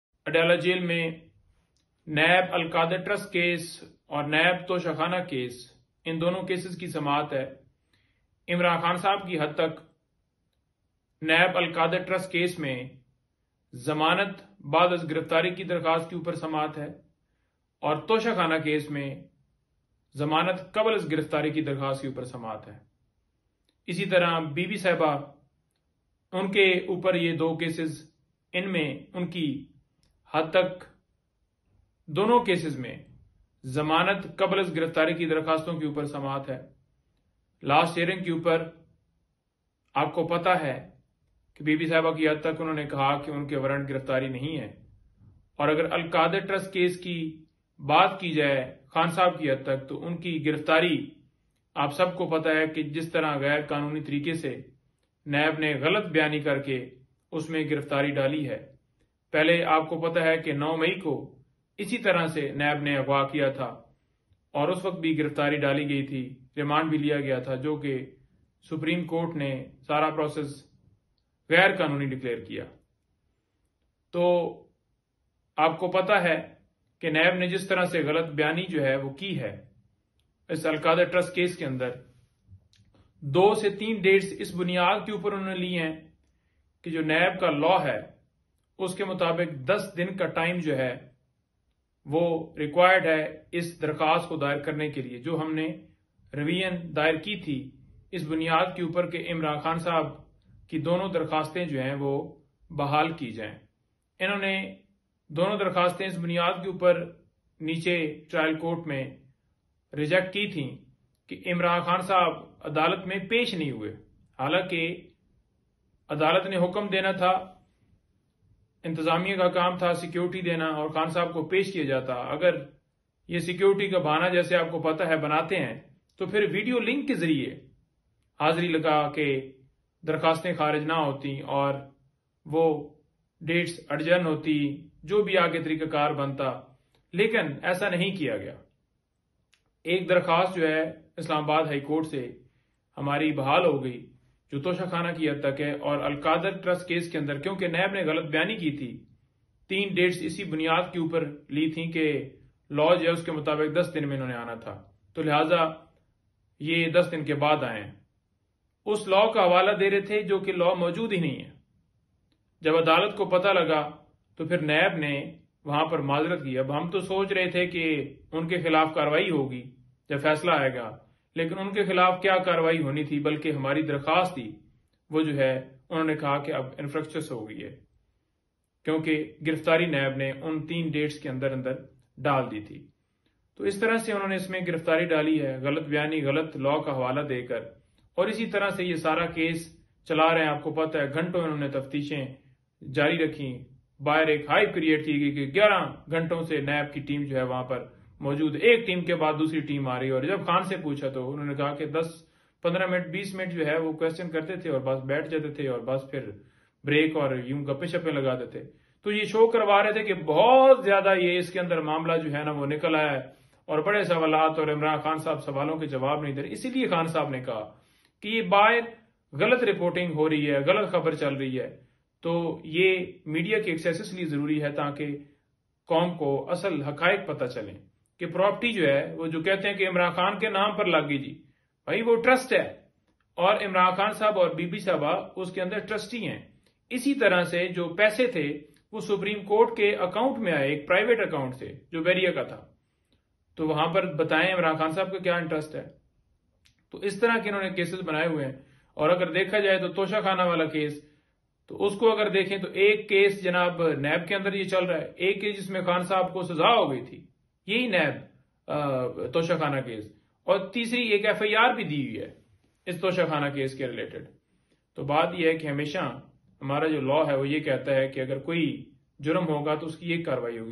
Big news regarding Imran Khan's case, Naeem Haider Panjuta issued an emergency statement
عمران خان کے کیس کے حوالے سے بڑی خبر نعیم حیدر پنجوتہ نے ہنگامی بیان جاری کر دیا